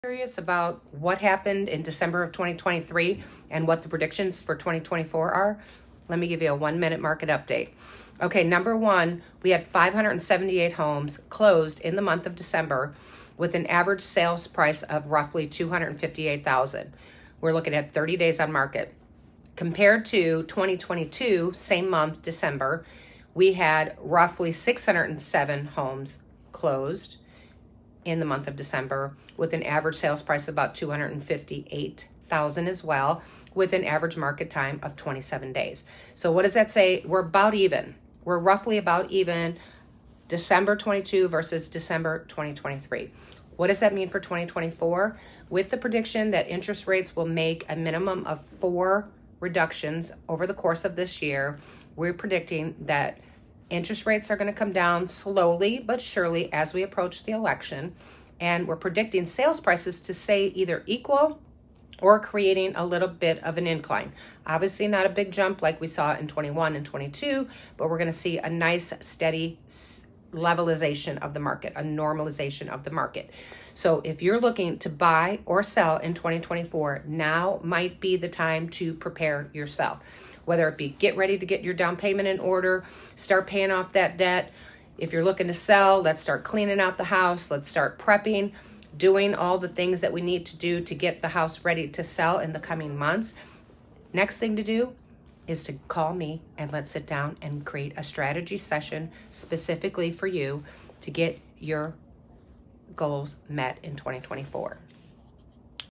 Don’t move to Central Arkansas till you watch this video 2024 market prediction
Central Arkansas 2023 final market update and prediction for 2024 prediction for Real Estate in Little Rock and metro surrounding market